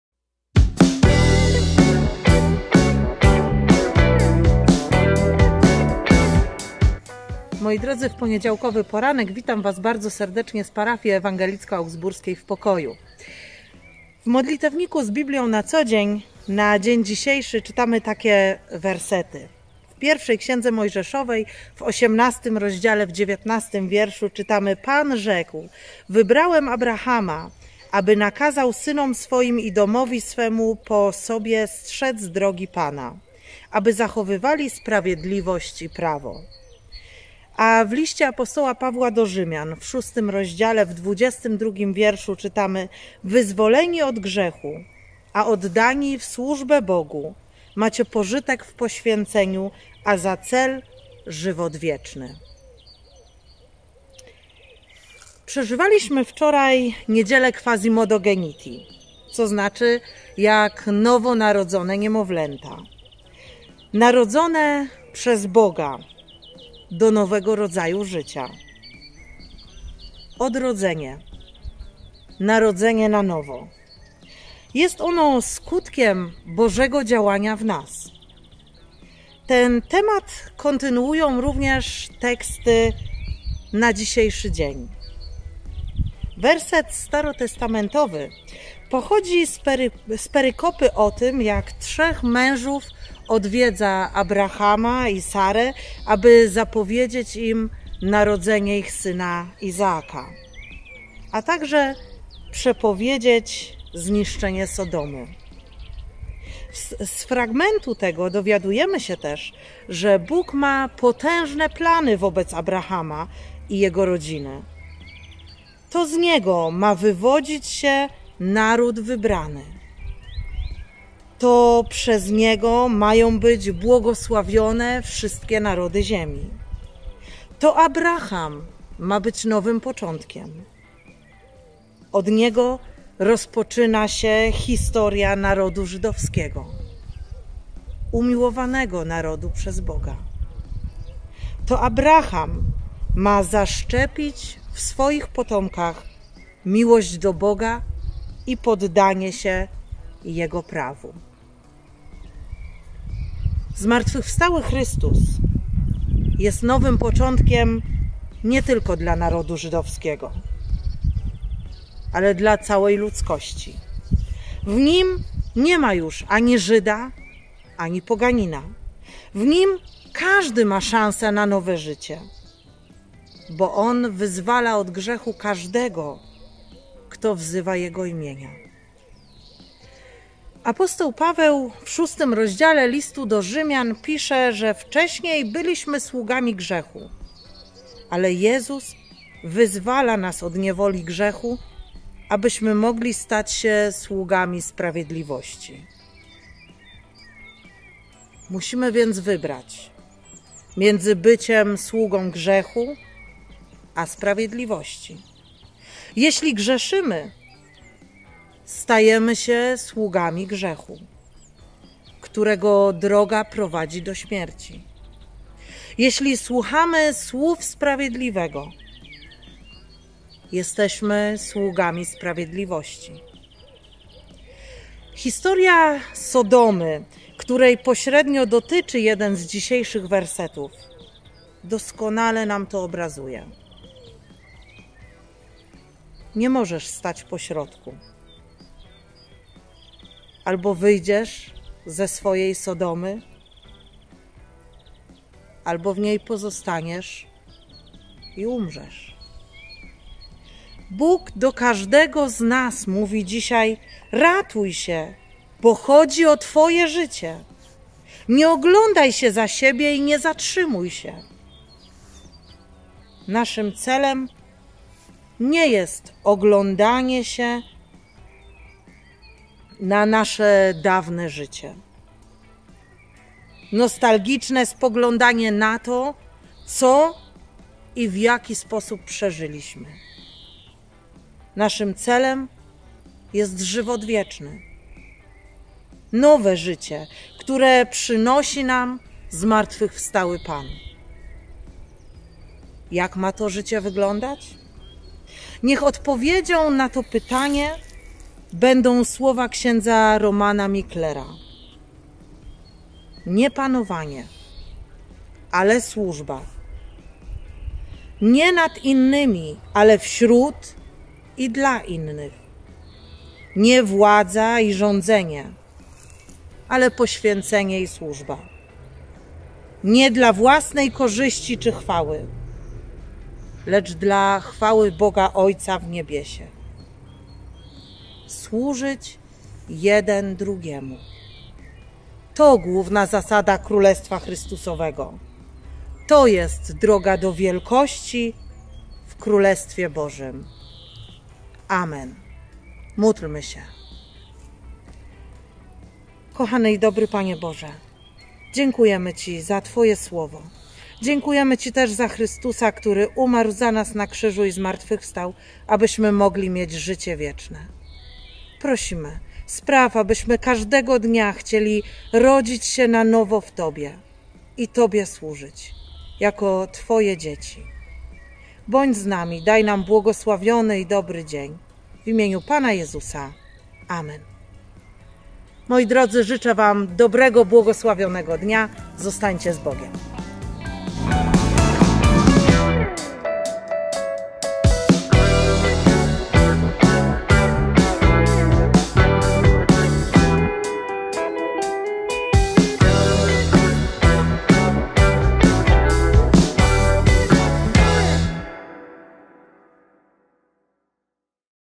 Codziennik biblijny, Słowo na dzień 20 kwietnia 2020 r.
Katarzyna Kowalska, parafia ewangelicko-augsburska w Pokoju.